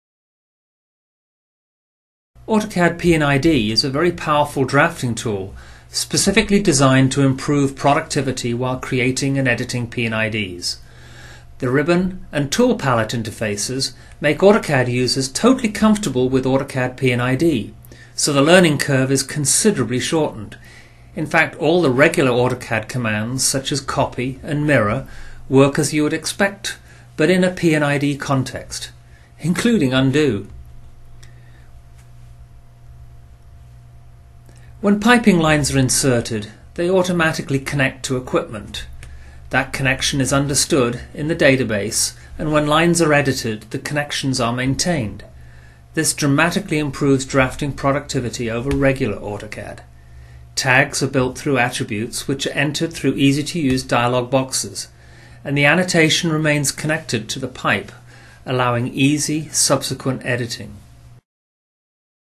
Autodesk Plant Design Suite 2014 P&ID & piping design - Create and manage P&IDs
Autodesk® AutoCAD® P&ID software enables you to more easily create P&IDs.